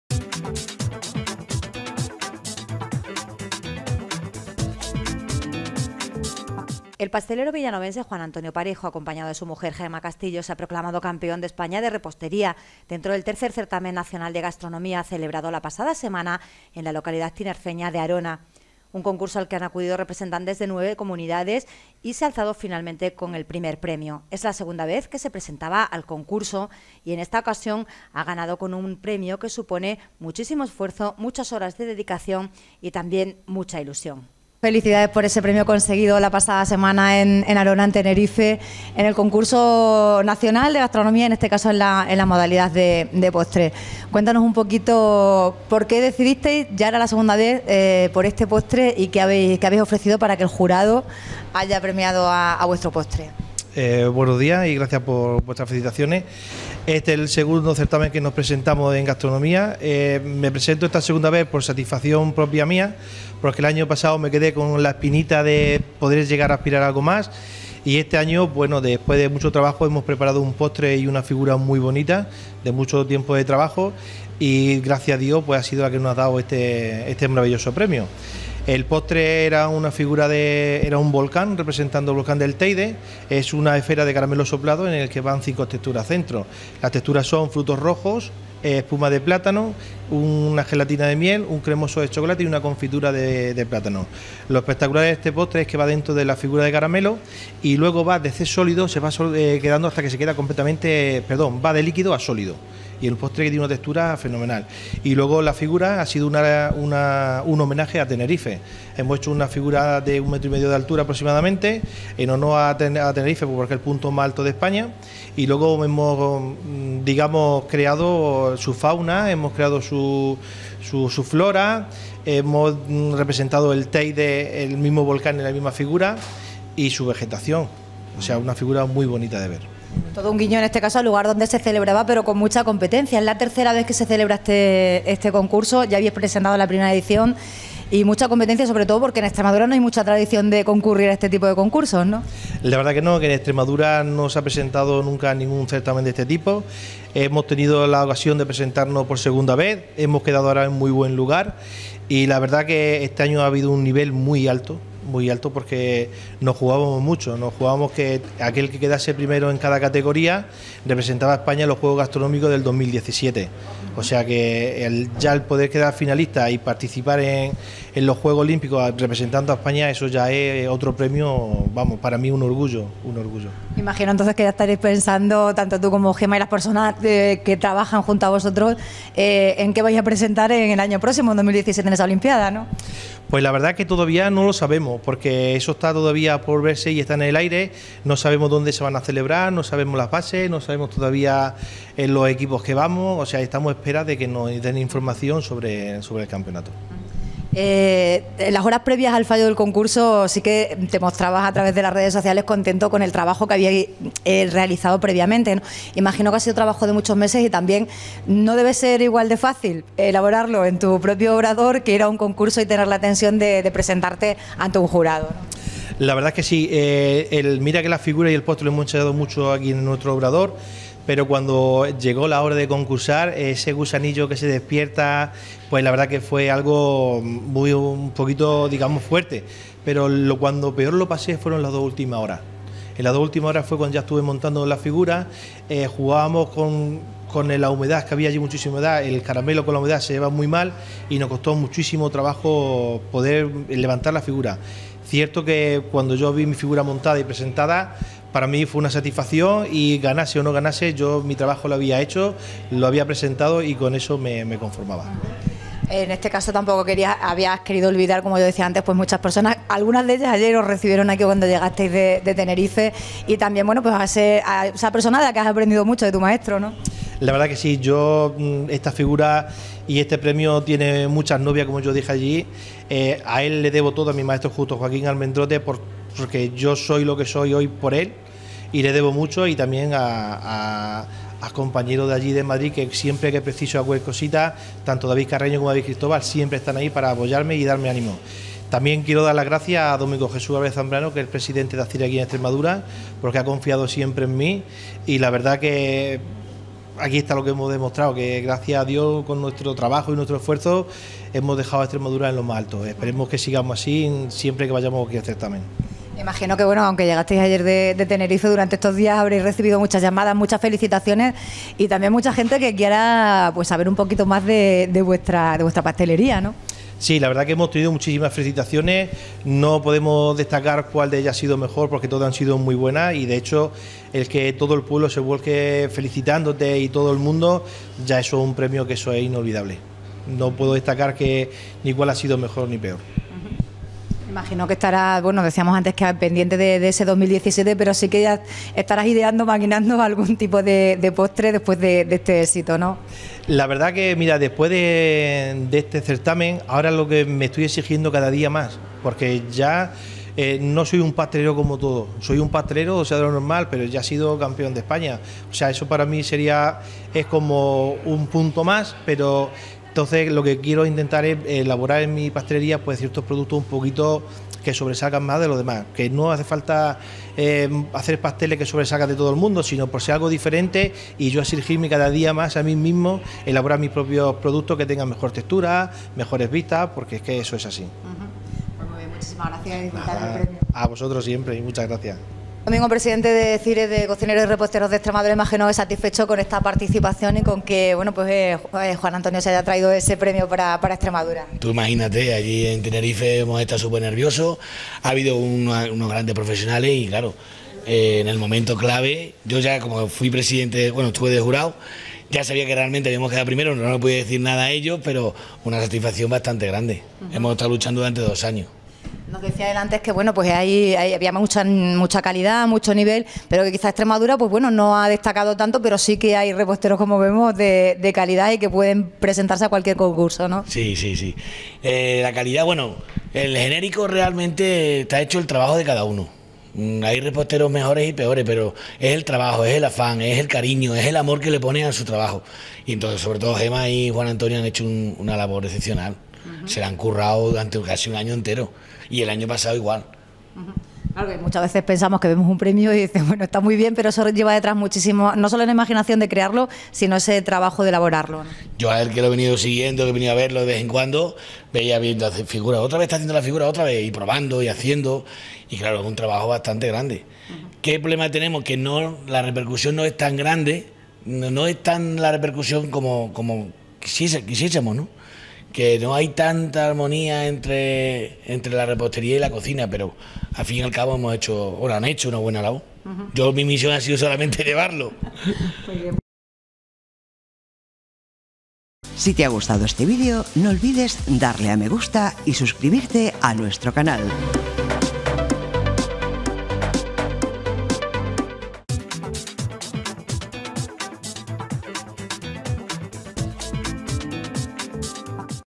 Juan Antonio Parejo, de Pastelería La Florentina, campeón de nacional de repostería
Con un postre y figura en el que lleva trabajando meses

El pastelero villanovense Juan Antonio Parejo, acompañado de su mujer, Gema Castillo, se ha proclamado campeón de España de repostería, dentro del III Certamen Nacional de Gastronomía;  que se ha celebrado la pasada semana en la localidad tinerfeña de Arona. Un concurso al que han acudido representantes de nueve comunidades. Además de Extremadura, han estado representadas Navarra, Cantabria, Aragón, Castilla la Mancha, Castilla y León, Baleares, Canarias y Madrid.

Es la segunda vez, el año pasado quedó en segunda posición, que el propietario de la pastelería La Florentina  se presentaba a este concurso. En el concurso era obligatorio presentar un postre compuesto no sólo por el propio postre sino también por una figura, que en este caso la elaborada por Juan Antonio y Gema medía aproximadamente 1,5 metros. Esta figura artística era de estilo libre, pero sí tenía como requisito el que estuviera elaborada con chocolate y caramelo. El postre debía tener también como ingredientes obligatorios el plátano y la miel.

Este postre, en el que han estado trabajando durante meses, tenía una larga elaboración. De hecho, comenzaron a las 10.30 horas de la mañana y concluyeron a las 15.30 horas. Uno de los principales problemas, recuerda Juan Antonio, ha sido la humedad que había. Un elemento nada adecuado para el caramelo o el chocolate.

En cuanto a las características del postre, Juan Antonio Parejo explica que era un volcán de plátano, miel y frutos rojos, homenaje a la tierra donde se ha desarrollado el concurso.  Todo ello con seis texturas: una salsa de frutos rojos, espuma de plátano, cremoso de chocolate, confitura de plátano, una gelatina de miel y un strussel de chocolate. La complicación del postre además de la humedad que había, añade; es que el postre va dentro de una esfera de caramelo soplado.
Por lo que respecta a la figura, Parejo expresa que ha querido hacer un homenaje a la isla de Tenerife, que tiene al volcán del Teide el punto más alto de España. Con esta figura ha querido representar esta isla, su flora, su fauna marina, sus acantilados ...
Aunque Juan Antonio lleva muchos años en el mundo de la panadería y repostería, a los 15 años empezó a trabajar en una panadería y desde hace cinco se dedica a la pastelería de forma plena, poniendo en marcha su obrador hace 4; reconoce que en este postre ha trabajado meses.  Además de imaginarlo, lo difícil es precisamente darle forma y hacer realidad lo que uno se imagina.
Juan Antonio Parejo sabe que este premio no sería posible sólo con su trabajo, que ha sido mucho. Importante es, explica, el apoyo en todos los sentidos de su mujer, Gema Castillo. Además, están detrás muchas personas. Por supuesto su familia, pero también tiene muy presente el apoyo incondicional de su maestro, Justo Joaquín Almendrote, al que dice le debe todo lo que es en este apasionante mundo.

No menos importante es el apoyo que ha tenido por parte de Domingo Álvarez, presidente de la Asociación de Cocineros y Reposteros de Extremadura (Acyre). Confió, indica, "en mi para que estuviera en el equipo regional que concursaba".
Este premio es muy importante para él, para su trabajo y esta pasión que tiene y siente por la repostería. Este premio, el máximo reconocimiento a nivel regional, ya le está deparando muchas satisfacciones. Entre ellas, el que estará con el equipo nacional en los juegos olímpicos de gastronomía que se celebran el próximo año.  Más inmediata es su presencia en diferentes certámenes gastronómicos tanto en nuestra región como fuera.

Aún disfrutando de este premio, de la ilusión que les ha hecho y la recompensa por el trabajo hecho; Juan Antonio sigue con los pies en la tierra y nada más llegar de Canarias, ha vuelto a su trabajo diario: a su obrador. En él continúa elaborando una de sus especialidades, las florentinas. Un dulce elaborado básicamente con almendra pero con una amplia gama de ingredientes variados. Otras especialidades son las pastas de té, las tejas de almendra y, por supuesto, las tartas. Sin olvidar otros dulces típicos de diferentes épocas del año como los huesos de santo, buñuelos, turrones artesanos al corte, dulces estivales o roscones de Reyes.